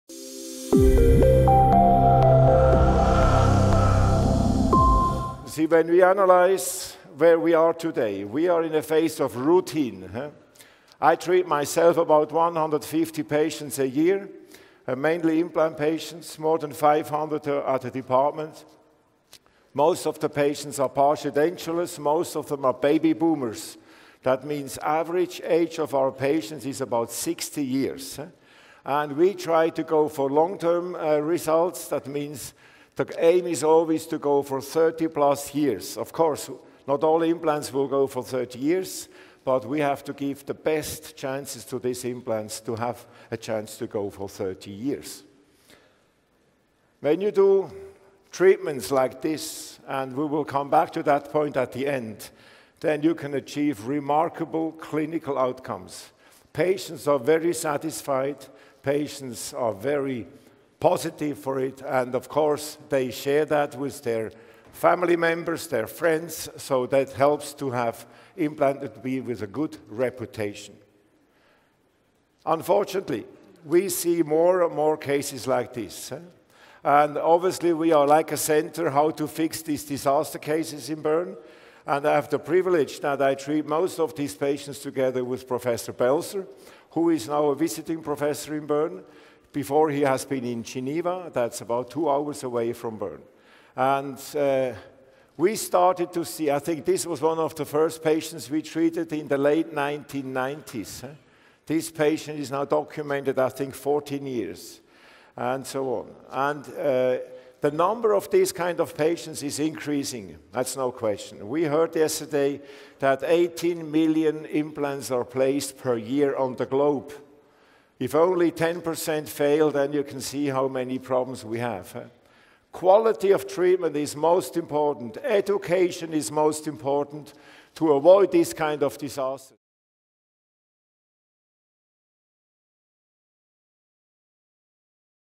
Management of Esthetic Implant Failures – Surgical Aspects | Daniel Buser
ITI Academy Congress Lecture Preview: Management of Esthetic Implant Failures – Surgical Aspects by Daniel Buser.